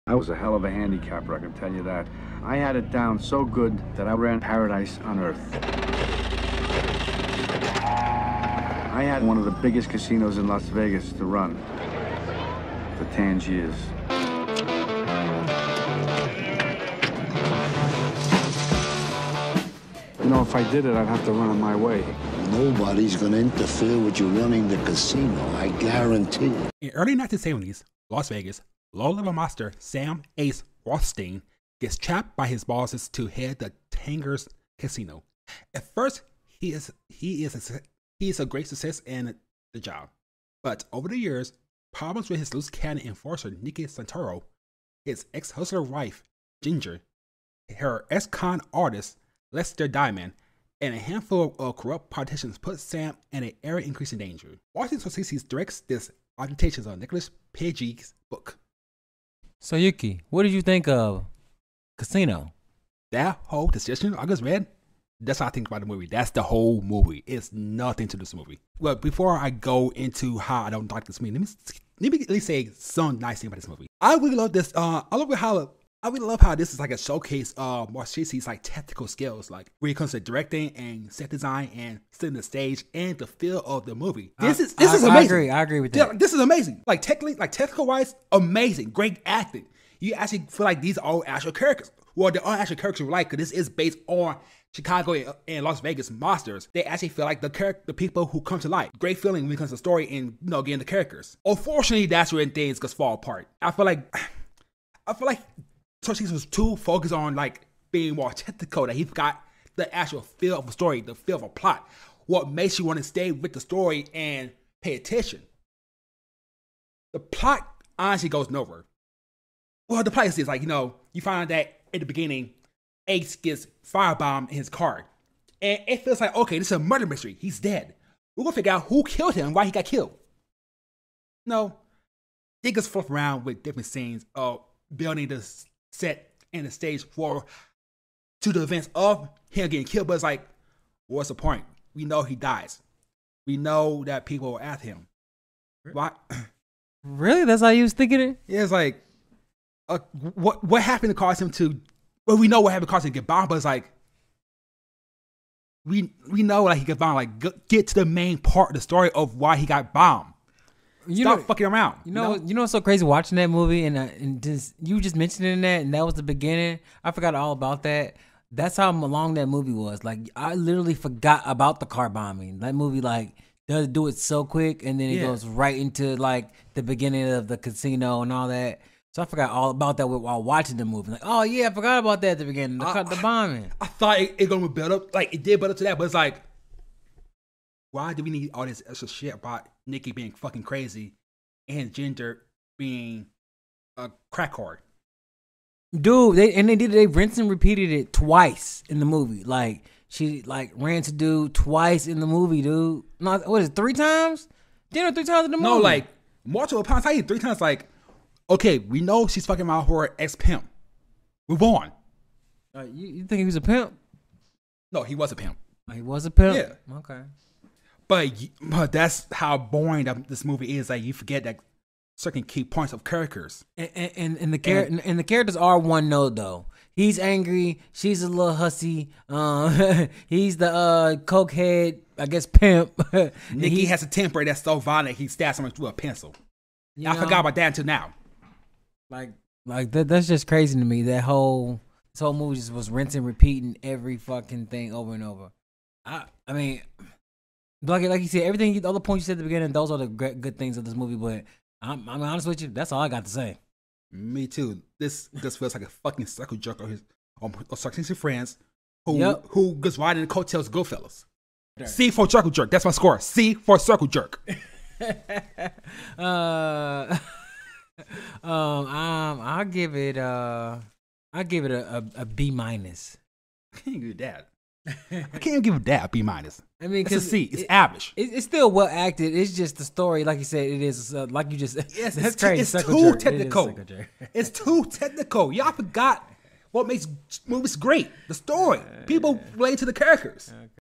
"Now you're gonna have to learn with your left hand" | Casino (1995) review | B side # 94
In this B side, Yuki and Tv Guru review Martin Scorsese's 1995 classic Casino.

 Find us @

 Hit us up at